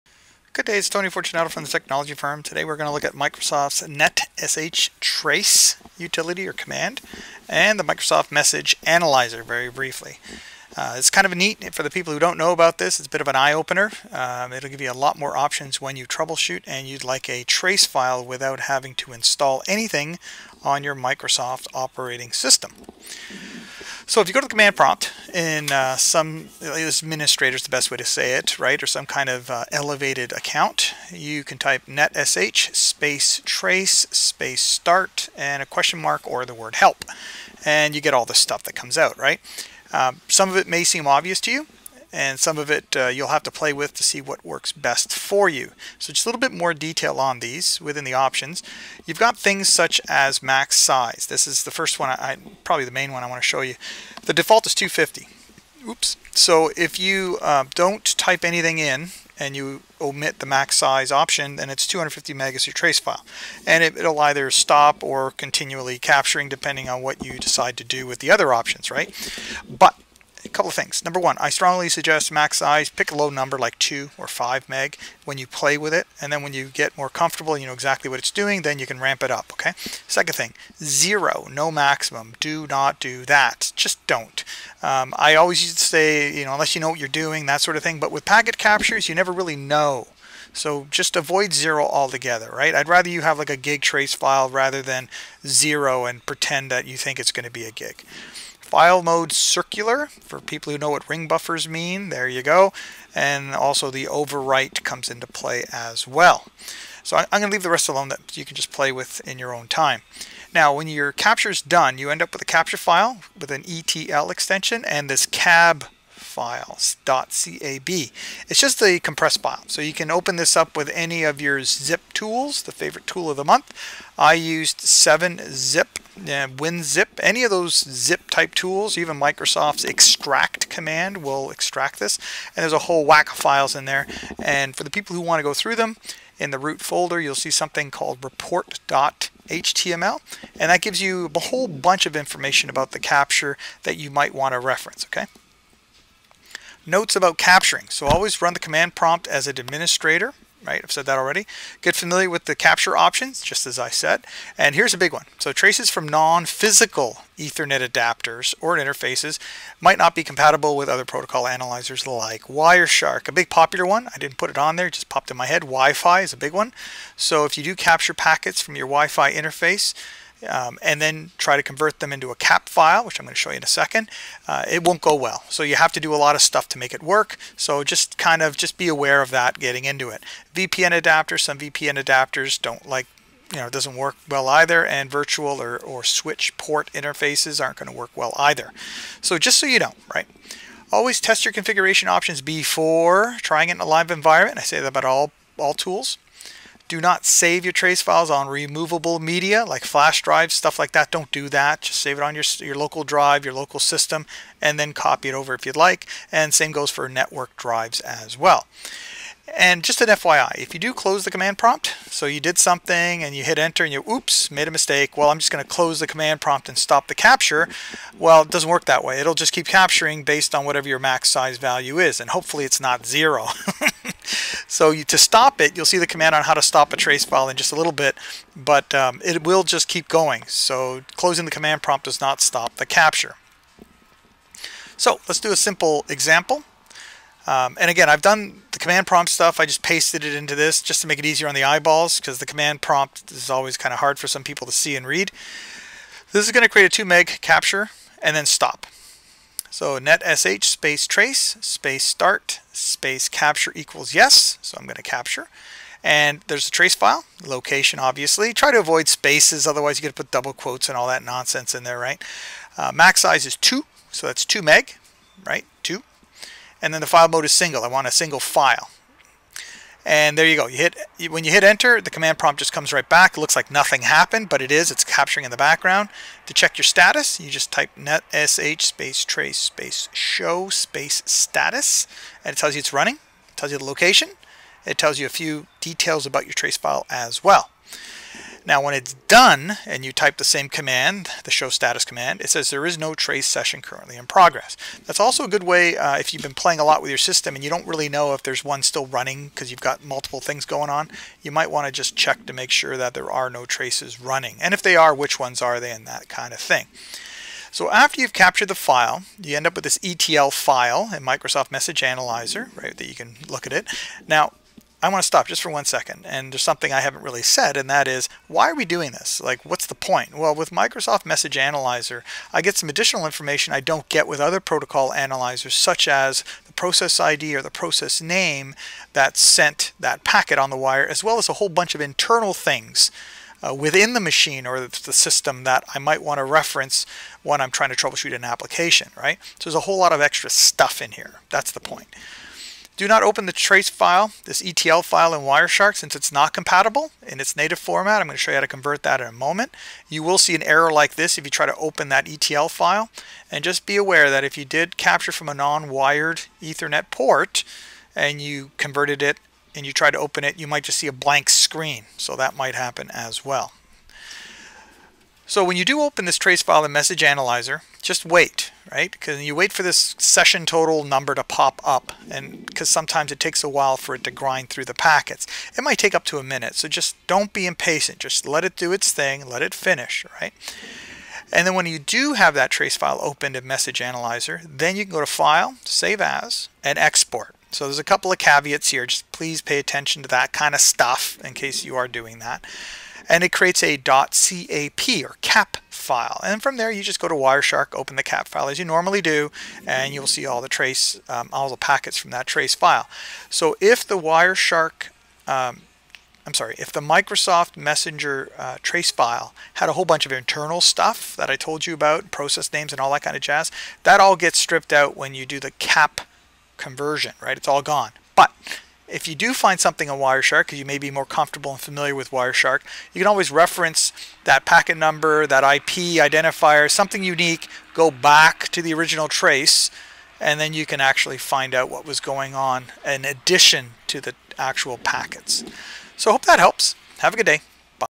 Getting Started With Microsofts netsh packet capture
Getting Started With Microsoft’s netsh
There are many challenges we face when I want to capture packets while troubleshooting.
- Installing a packet capture tool such as Microsoft’s Netmon or Wireshark, might be a deal breaker for some admins.
- Using a span or mirror port might not be available or add excessive latency to packets.
In most cases I would be happy with a solution that simply captures the packets and I can analyze the data on another system.

when its posted